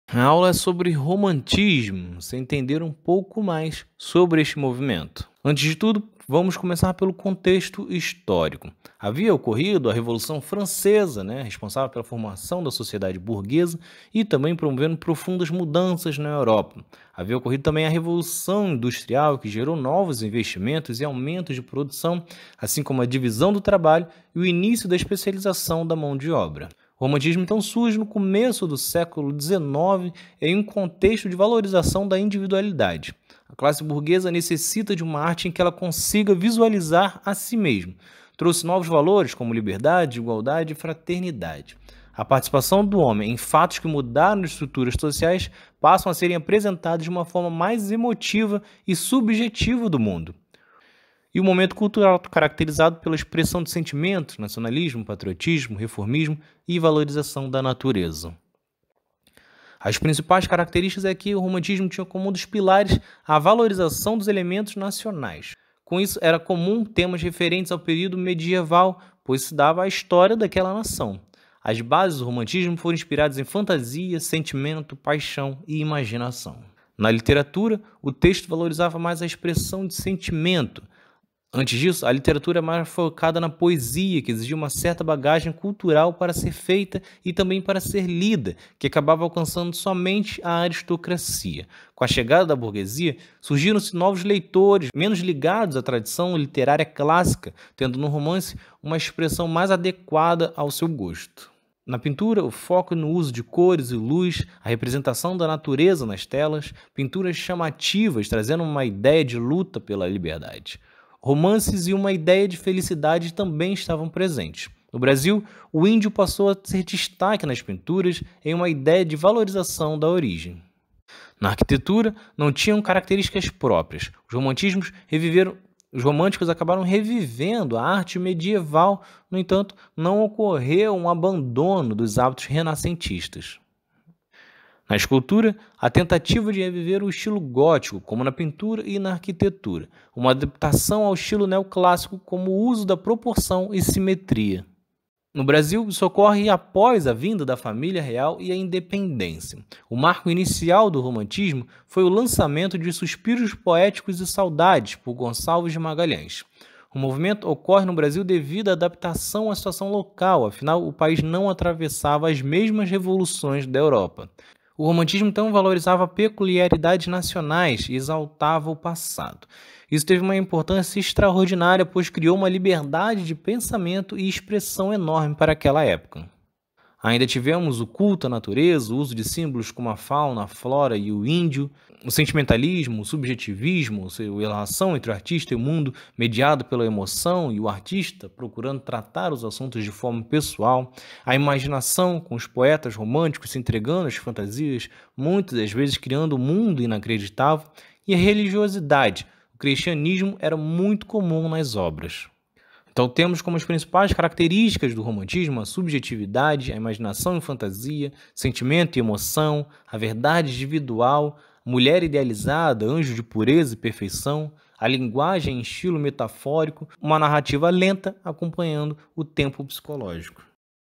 Aula de história resumida I Romantismo
Além do conteúdo que você já conhece do canal, mostrando o lado não contado da nossa história e das informações políticas, disponibilizarei aqui apresentações de temas de salas de aula.
Neste episódio tudo sobre o Romantismo: Contexto histórico, ideia, e formas na pintura, escultura, arquitetura e literatura.